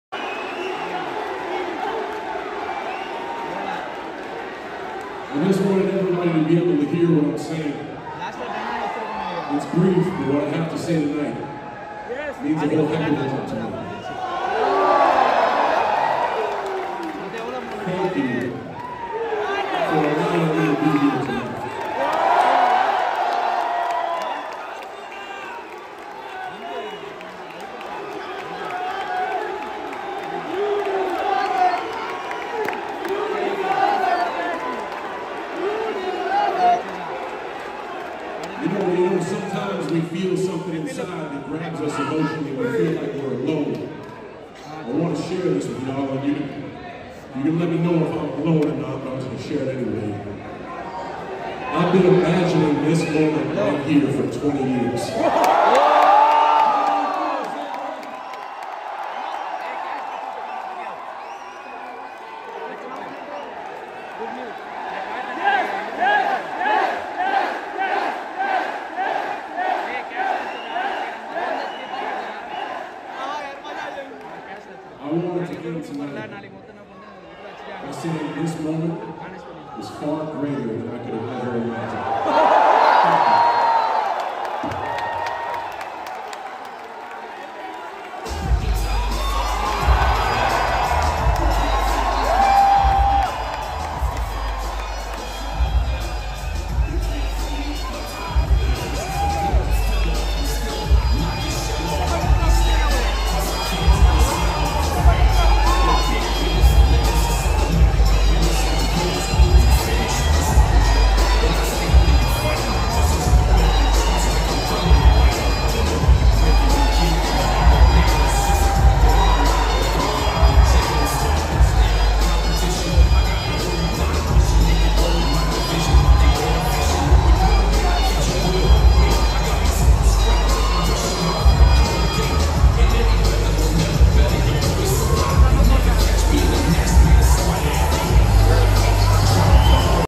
An emotional John Cena telling how he was waiting to wrestle in India since his debut ❤️🥹
JOHN CENA emotional Promo 🥹 First Home @WWE and 2nd Home India Bharat @JohnCena WWEHyderabad WWESuperstarSpectacle @wrestle_chatter @3dxchetan